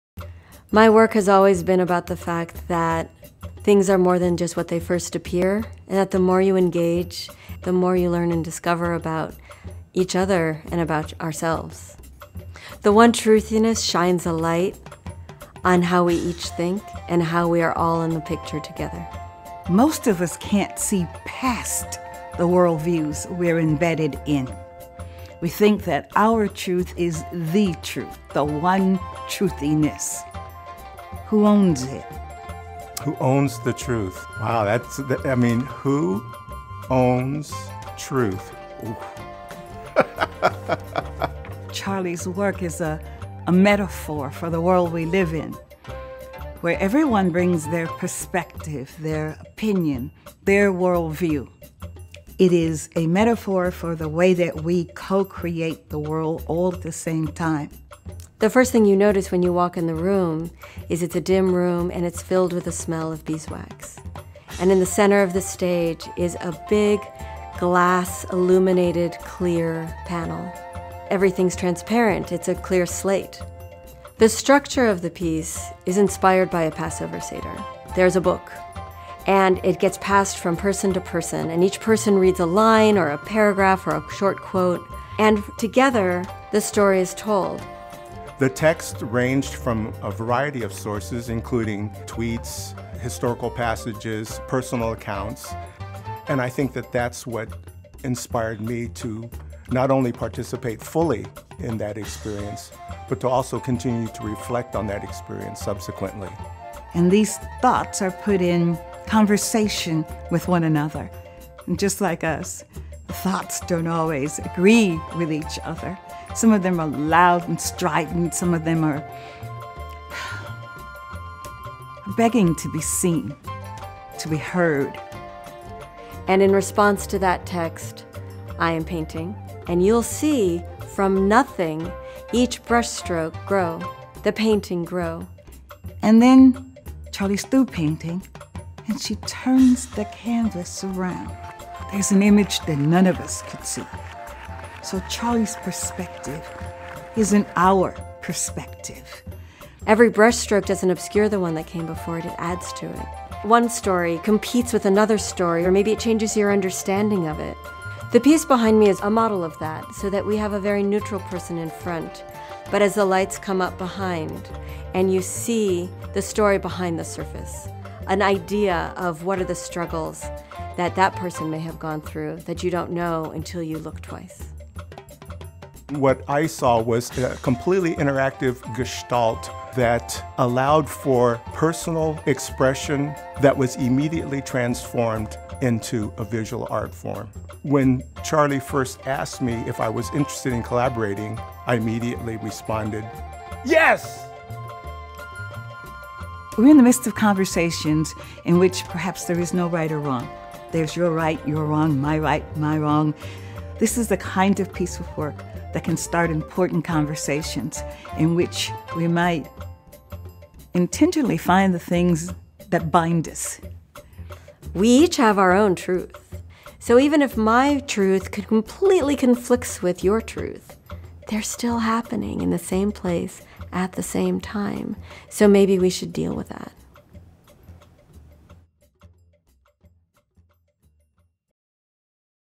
The One Truthiness 2017 — Trailer
An immersive experience of art, theater, and social engagement exploring how all our individual perspectives collide — a live painting in wax on glass is created in response to a text compiled of quotes drawn from the local community, Facebook, and Twitter, read by the audience.